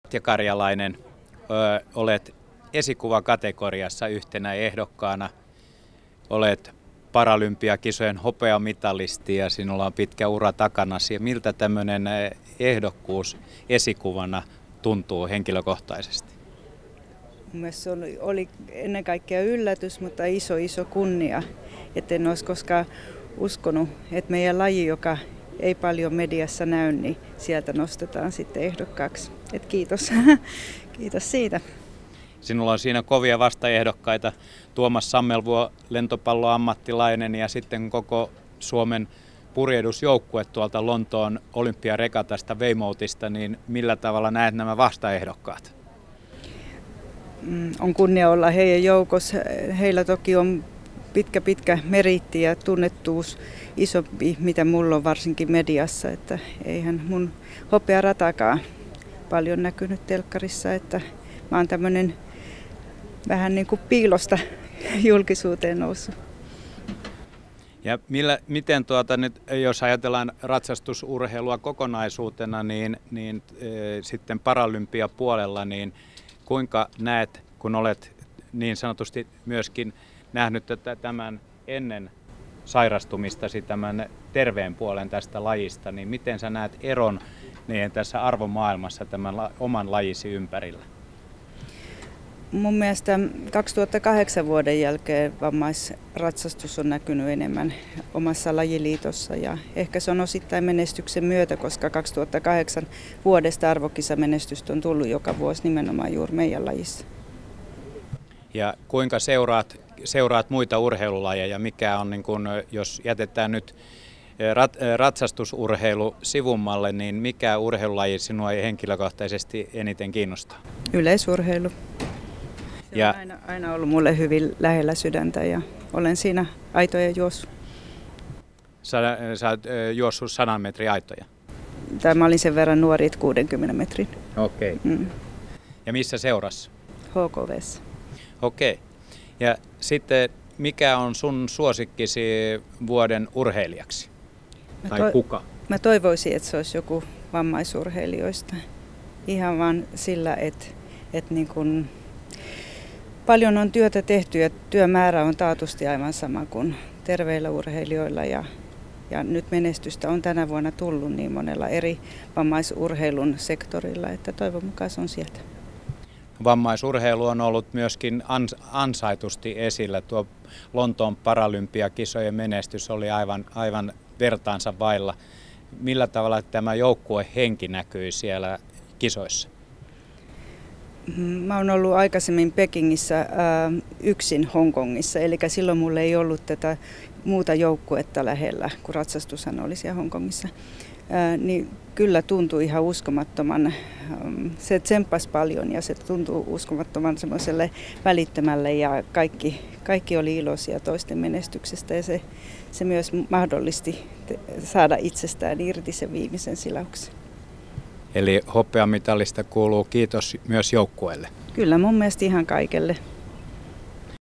Katja Karjalainen Esikuva-ehdokkaana Urheilugaalassa 2013
Paralympiahopeamitalisti Katja Karjalainen on Esikuva-ehdokkaana Urheilugaalassa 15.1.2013.

Urheilugaalan perustelut ehdokkuudelle: "Katja Karjalainen (s. 3.4.1962), aikuisiässä puhjennut vaikea sairaus ei ole estänyt urheilu-uran jatkumista kouluratsastuksessa ja nousua lajin kansainväliseen kärkeen, josta tuoreimpana näyttönä Lontoon paralympialaisten hopeamitali, vuorovaikutuskykyinen keskittyjä, paineiden hallitsija ja taistelija."

Muut Esikuva-ehdokkaat ovat lentopalloilija Tuomas Sammelvuo ja purjehduksen olympiajoukkue.